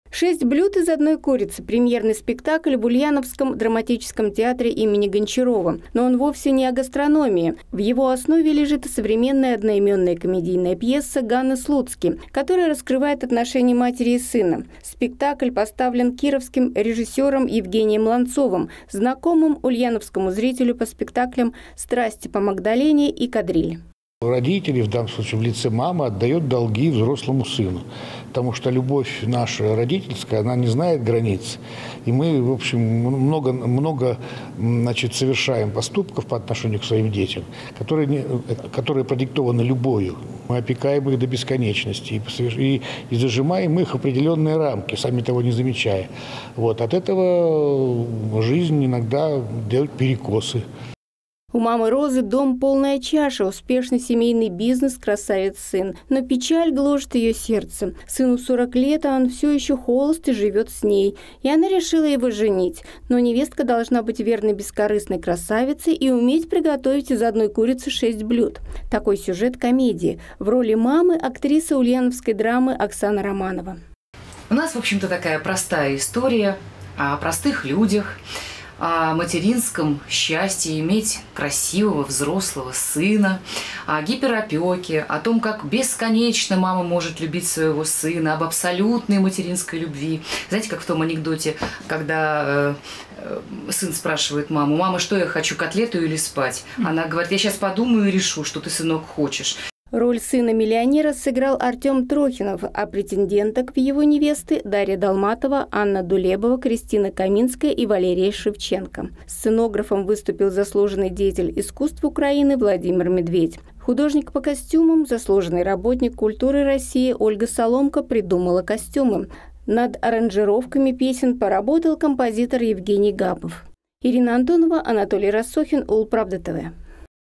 В ульяновском драмтеатре научат правильно женить сына и готовить шесть блюд из одной курицы
Ульяновский драматический театр имени И.А. Гончарова вновь порадовал  премьерой спектакля. На этот раз зритель увидел лирическую комедию. Подробнее о спектакле и о тех актёрах, кто в нём задействован, расскажет Ирина Антонова.